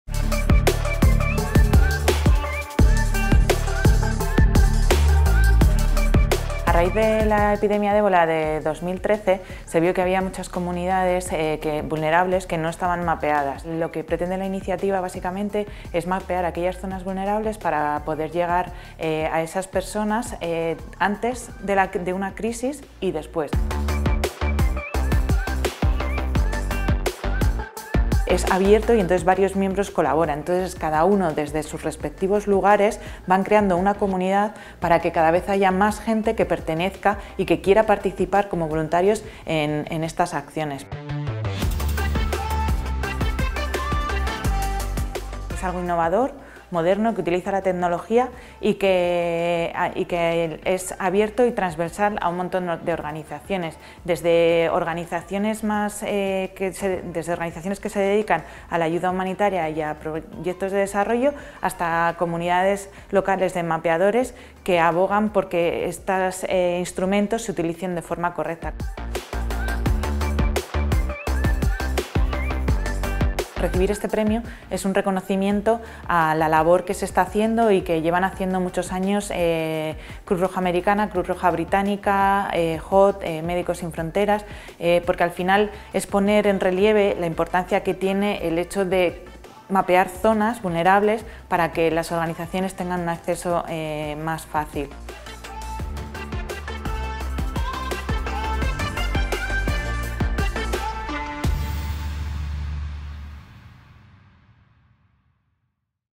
Premios Cruz Roja Tecnología Humanitaria: Missing Maps | Innovación
MissingMaps “Mapas Perdidos”, un proyecto humanitario promovido por diferentes organizaciones entre las que está la Cruz Roja de varios países, que se dedica a crear mapas detallados de las partes del mundo que son vulnerables a desastres naturales, conflictos armados y enfermedades epidémicas y donde las plataformas tradicionales no llegan.